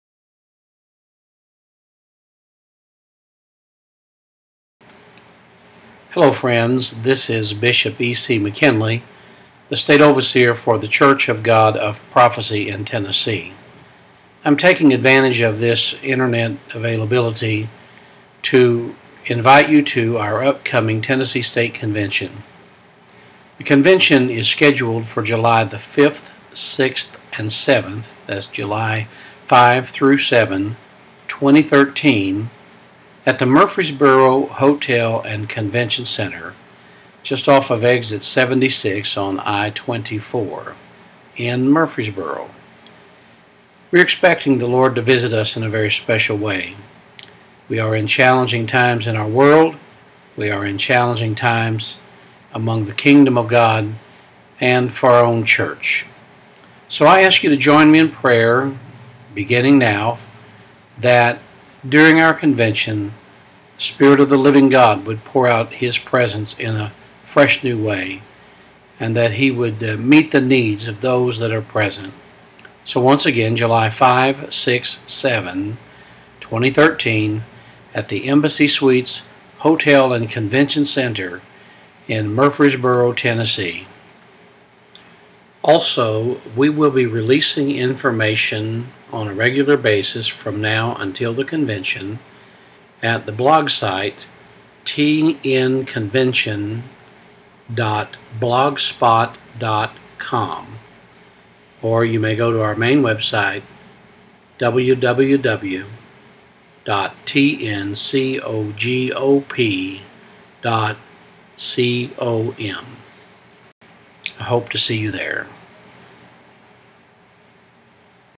2013 Convention Initial Invitaton
Announcing the 2013 Tennessee State Convention at the Embassy Suites Hotel and Convention Center in Murfreesboro, Tennessee.